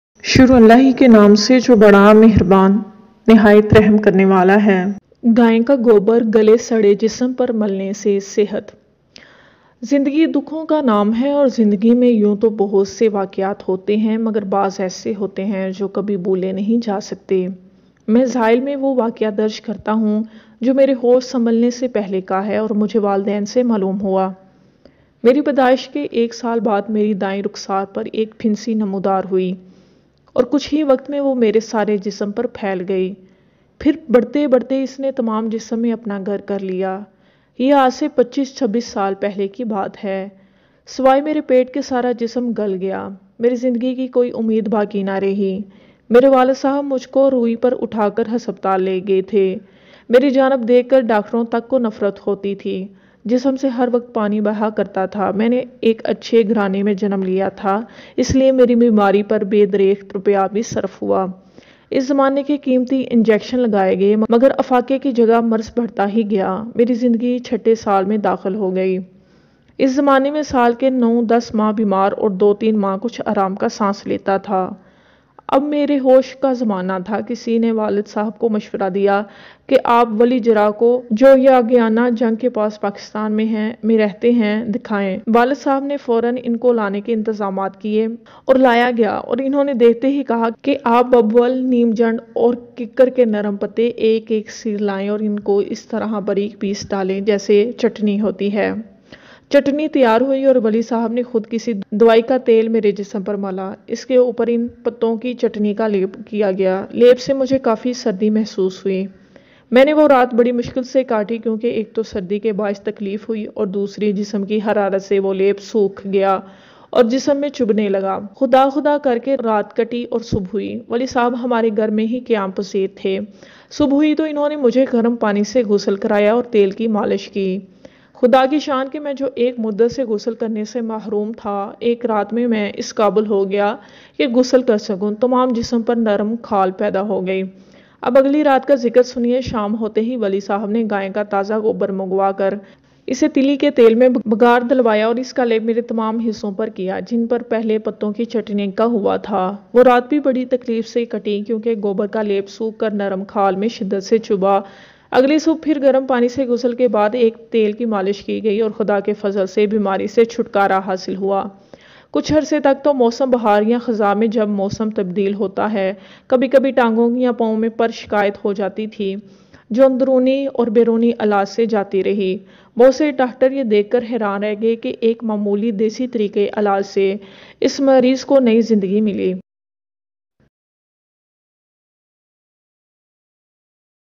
gay ka gober say galy jesum per malny say sahat | bimari se shifa ka tariqa in urdu
Gay ka gobar saygali jesum per malny say sahat | bimari se shifa ka tariqa in urdu